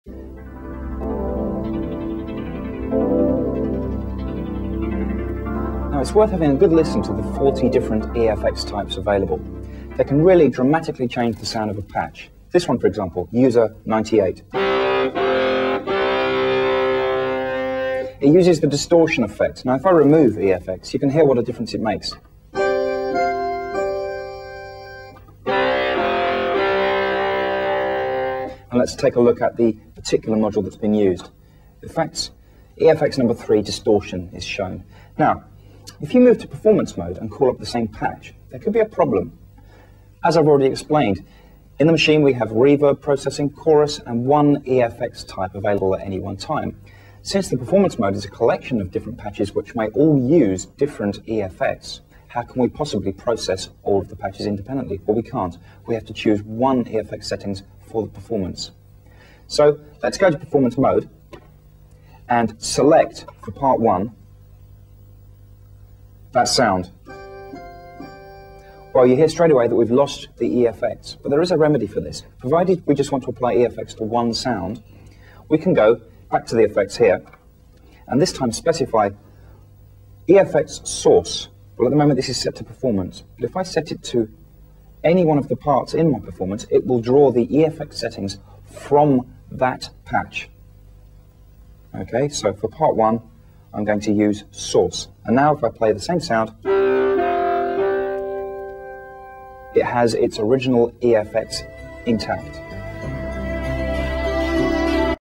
18-EFX_in_Performance-roland-xp80.mov
XP-60/80 Video Owner's Manual

which is a mine of usefule xp80 information. Nathan says "This manual is taken from XP-80 Video Owner's Manual. I had nothing to do with the production of the video and nothing to do with Roland (nor does Roland have anything to do with me)." They are such a great resource they should be widely available.